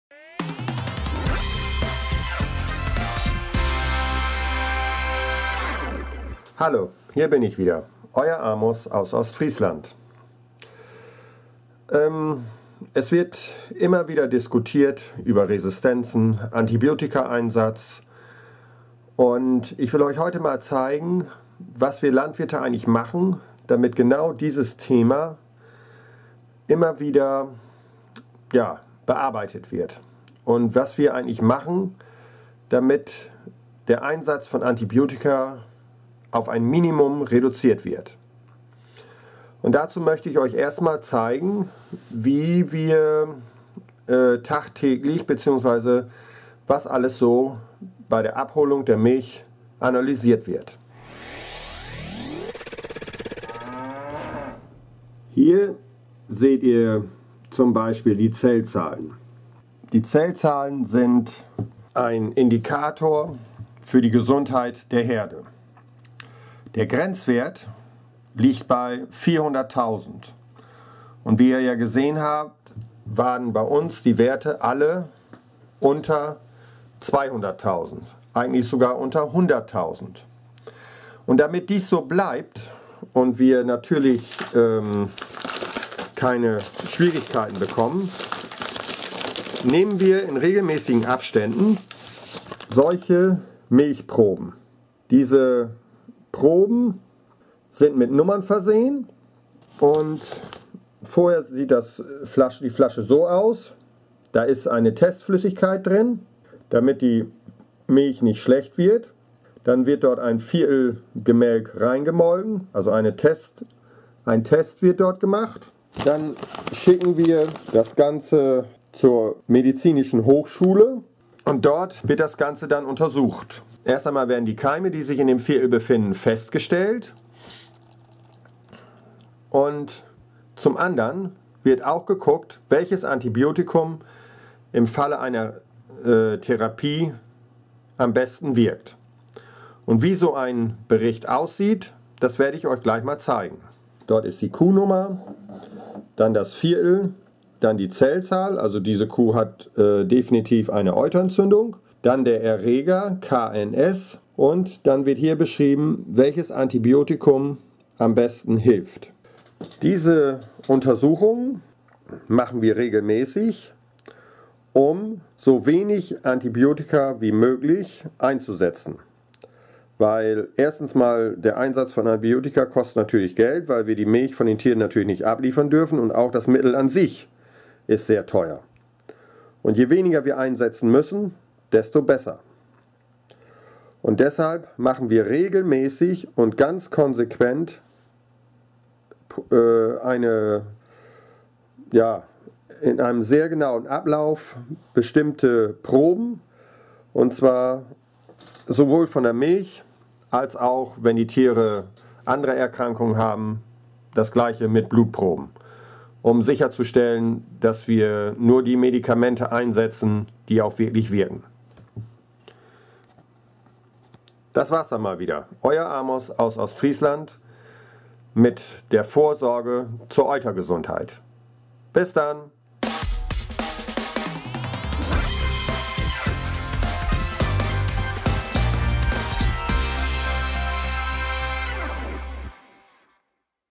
Antibiotika - My KuhTube Film 298
Auch die fittesten Kühe werden im Laufe ihres Lebens halt irgendwann mal krank. Das kennt man von sich selbst ja auch.
Und wenn man krank ist, dann möchte man - mit so wenig Medikamenten wie möglich - auch wieder gesund werden, weil
krank sein ja doof ist. Ganz normal, oder?! Und damit Antibiotika auch wirklich nur DA eingesetzt werden, wo sie wirklich unabwendbar
benötigt werden, wird sehr gezielt vorgegangen. Unser My KuhTube-Landwirt Amos aus Ostfriesland zeigt Einblicke in seine
Herdengesundheit und wie kontrolliert wird.